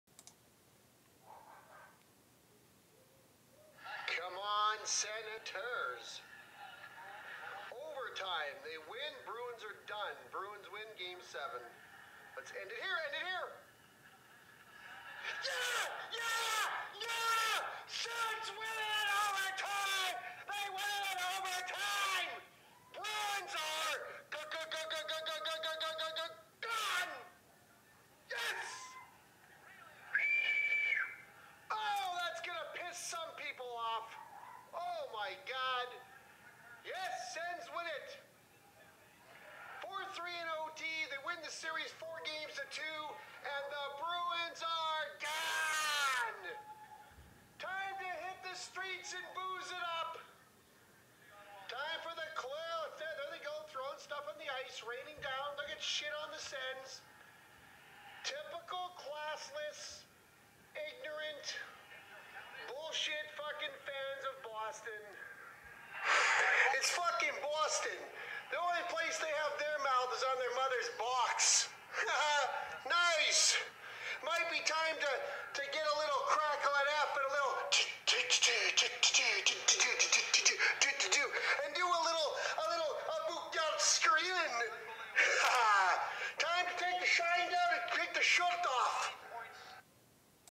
Senators @ Bruins - 4/23/17 - NHL playoffs - Game 6 - Overtime delight!!!!!!!!!!
Fuck the Bruins and their fans! Have a seat and enjoy the summer!!! Have a beer... Or many! 4/23/17.